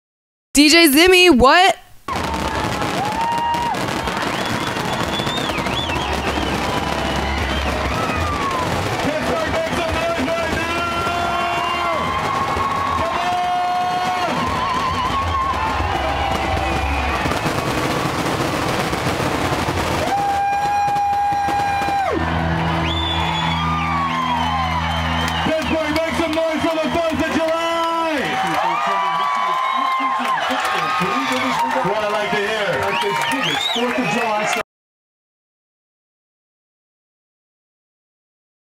DJ Zimmie - 4th of July 2011
View from the Red Bull MXT at Pittsburgh's Regatta.  Major.